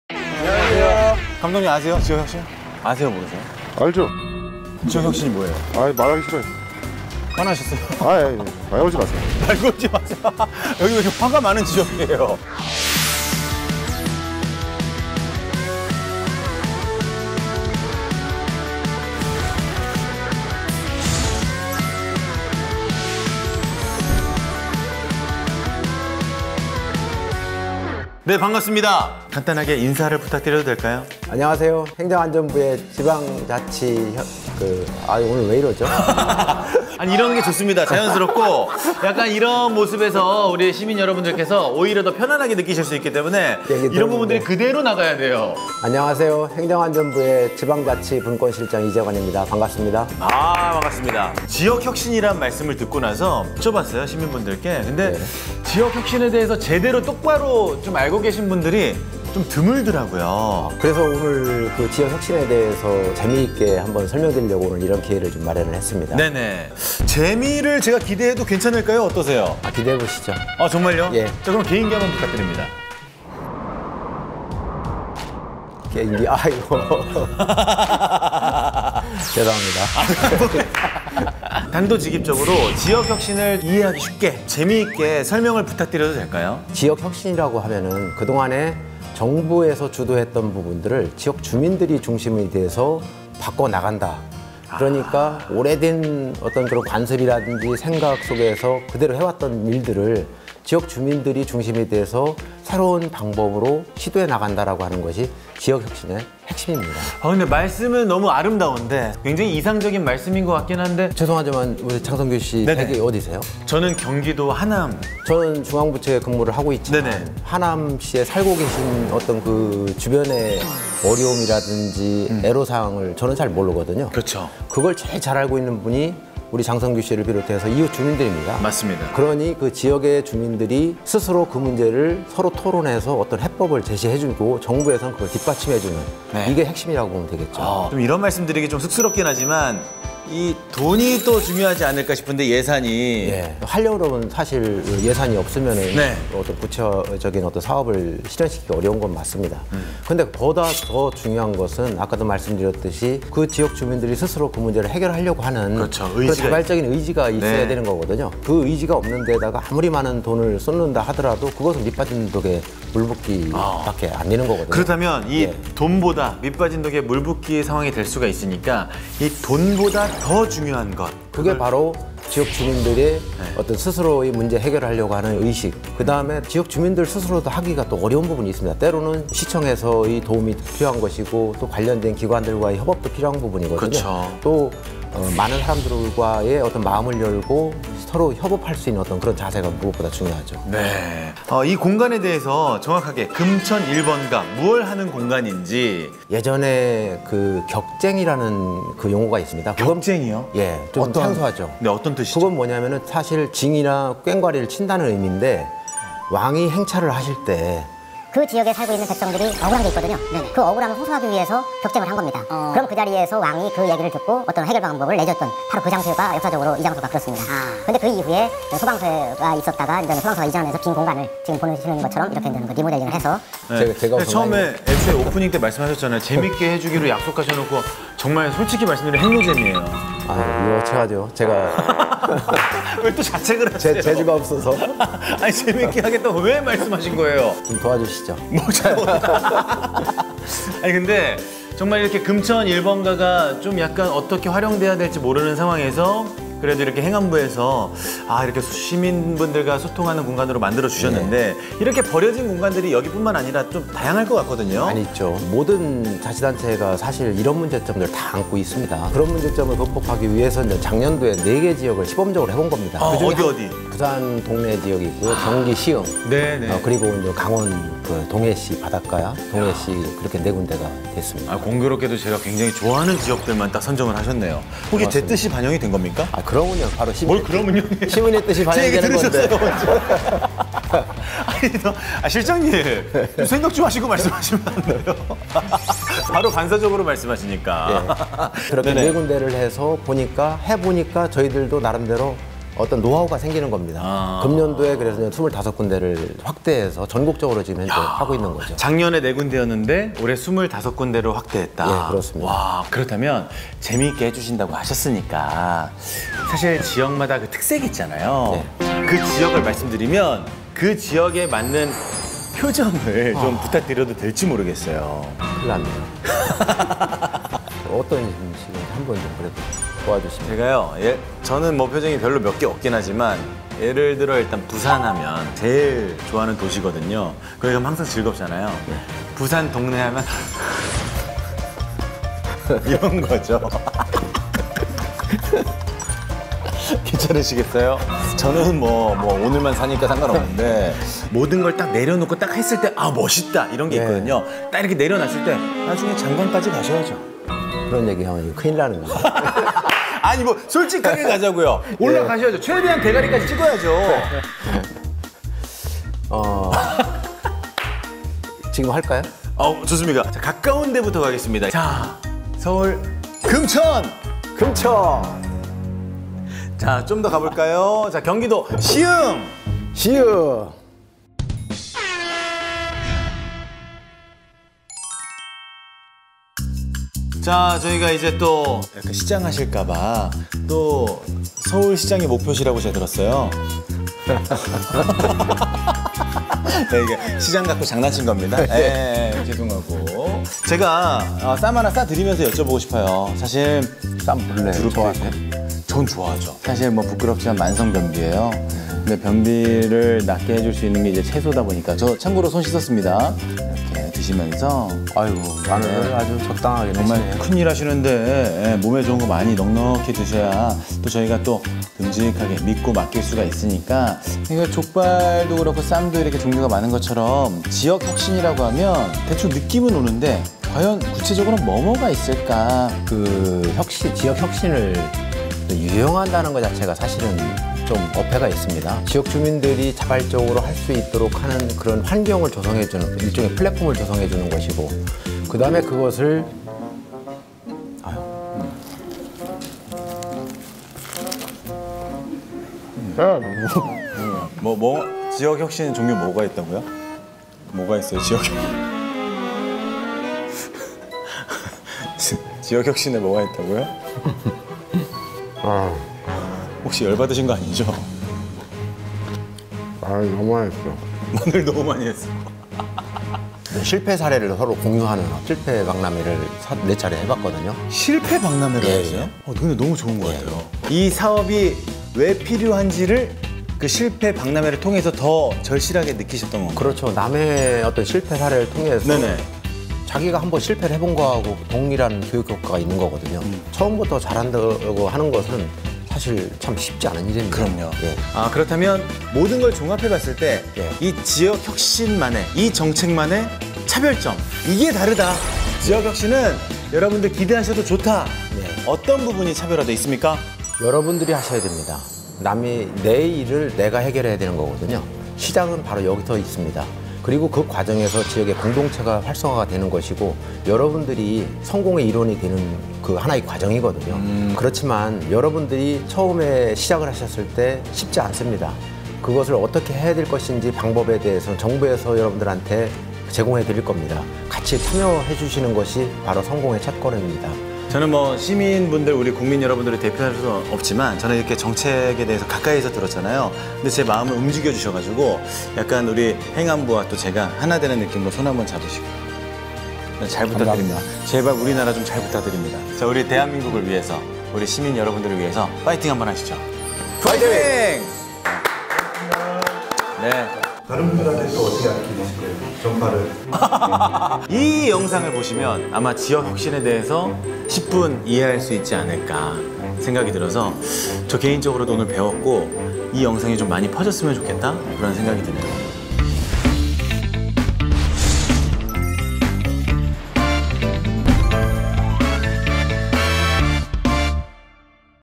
"지역혁신 이란?" 대세 장성규, 이 인터뷰도 살릴 수 있을까요?
지역혁신 1번가인 금천1번가에서
장성규와 행정안전부 공무원이 만났다!

하지만 이름부터 낯선 지역혁신.
자칫 '노잼'으로 흐르기 쉬운 이 인터뷰,
과연 장성규는 살려낼 수 있을까요?

극한 인터뷰를 지금 시작합니다!